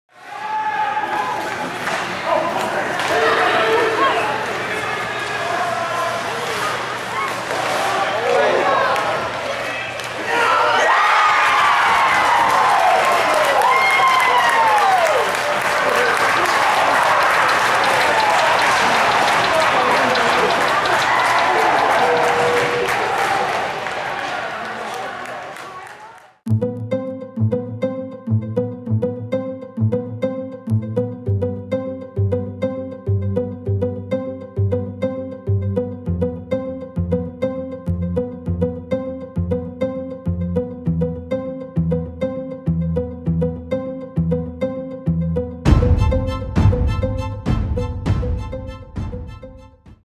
Alexandre Jacques Overtime Goal
2007 born Yale Hockey Academy U17 Prep forward Alexandre
Jacques (#17) scores in overtime to seal the victory over BWC Academy during CSSHL action.
Location - Burnaby Winter Club